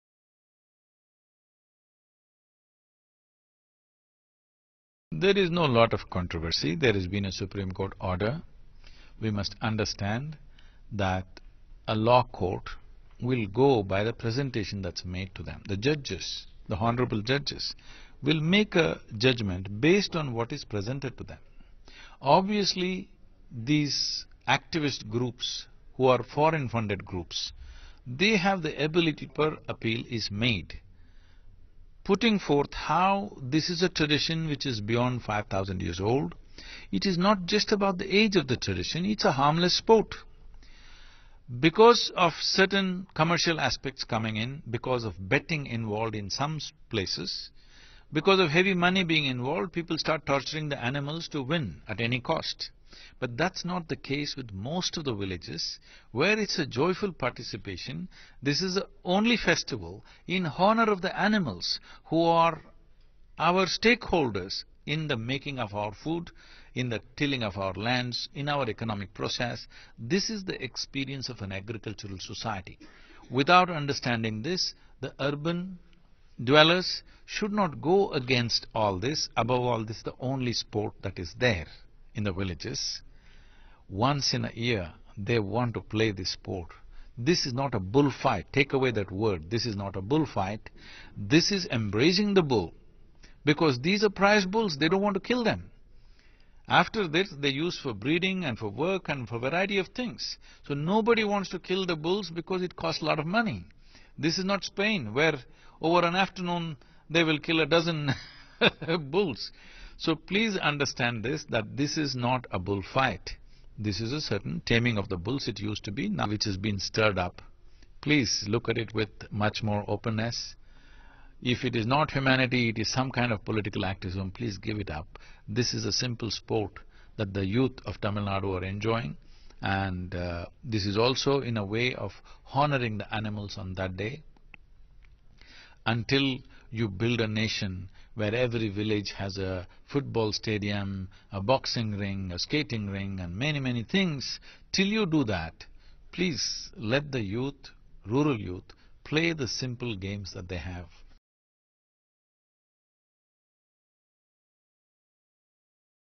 Jallikattu is Not Bull Fight,This Is Embracing The Bull, Says Sadguru
Jallikattu is Not Bull Fight,This Is Embracing The Bull, Says Sadguru Jaggi Vasudev.